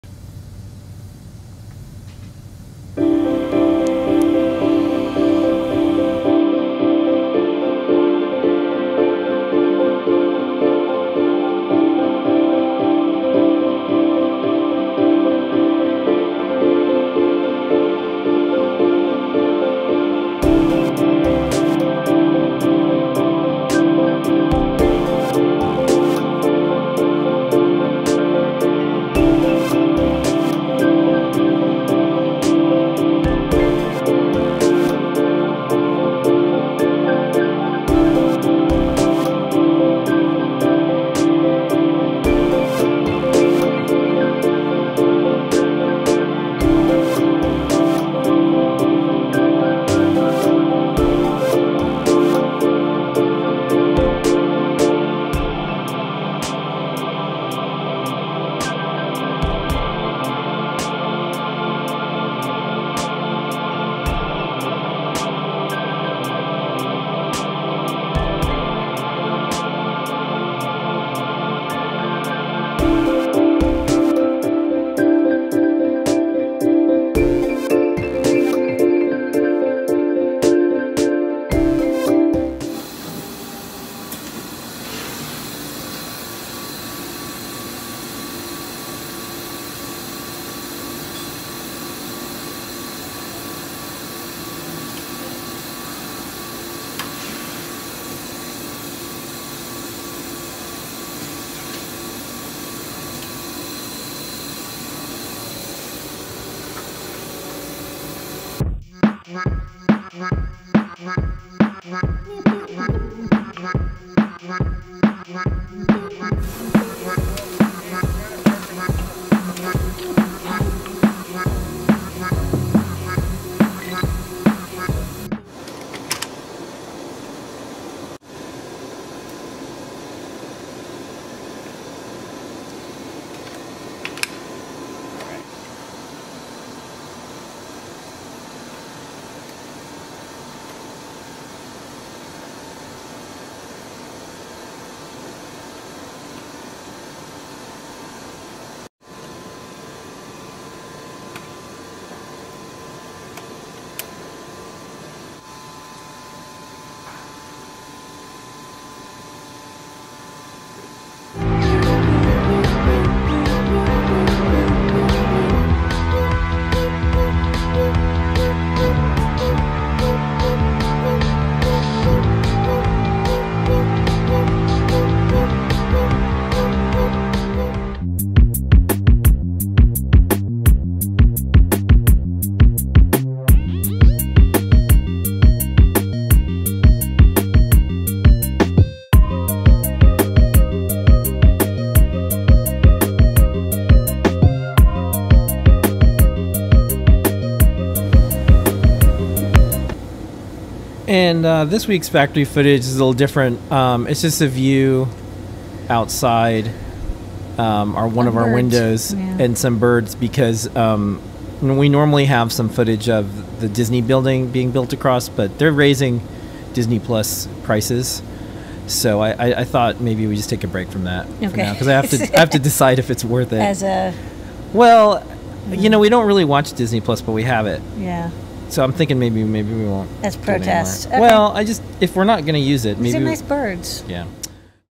Advanced Manufacturing 8/10/2022 Changing a Reel of Components
Board loader unloading a stack of Servo Shield PCBs (0:03)

Stencil taking in a blank panel for MPL3115A2 Sensors (1:23)

Various components taped & reeled (1:49)

Changing a reel of components (2:05)

Prepping products in pink baggies (2:42)

Assembling a tester for the Feather M0 Basic Proto (2:58)

View from the 10th floor looking south (3:21)